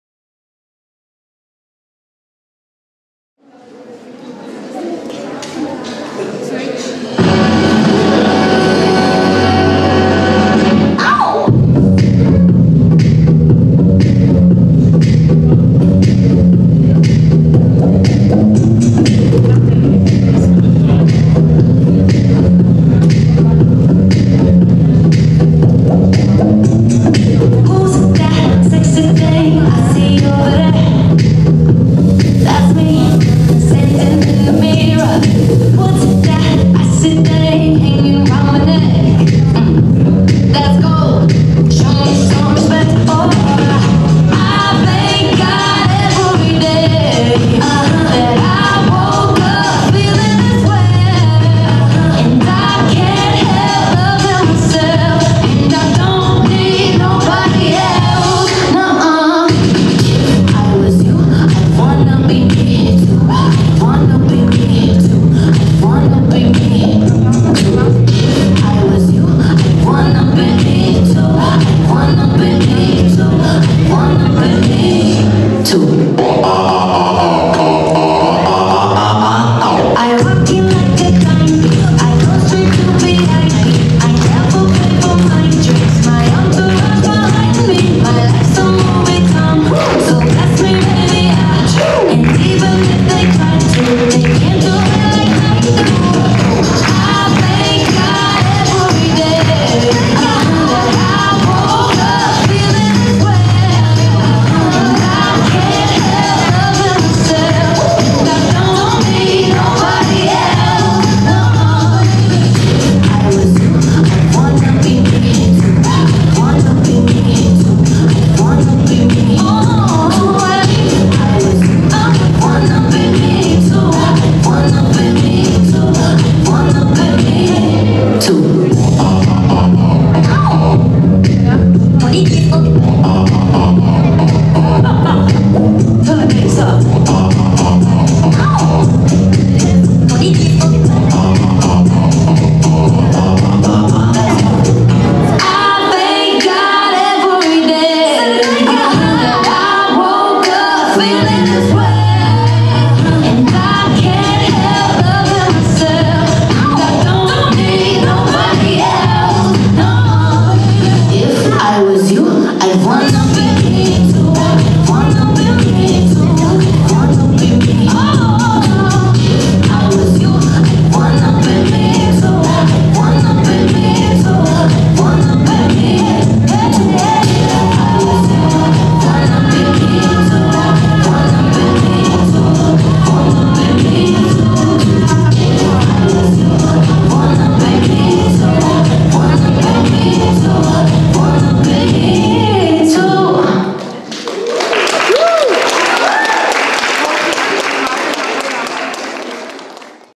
Two Cats - Volné sestavy 2017
Two Cats - Veronika Slámová, Nina Vavroušková
kategorie A
Taneční soutěž Volných sestav 2017
pořádá Active - středisko volného času Žďár nad Sázavou